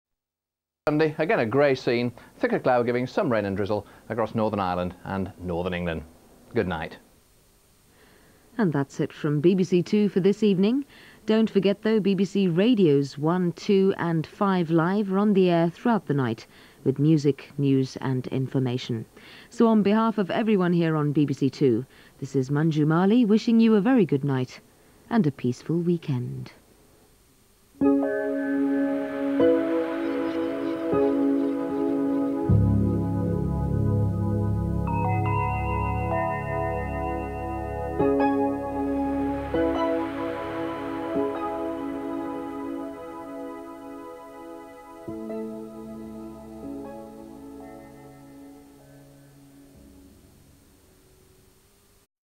Manju Malhi closes down BBC2 on Saturday 1st November 1997
BBC Announcer Manju Malhi closes down BBC2 on Saturday 1st November 1997. Manju has been a BBC TV announcer from 1990 to 1997 and again from 2001.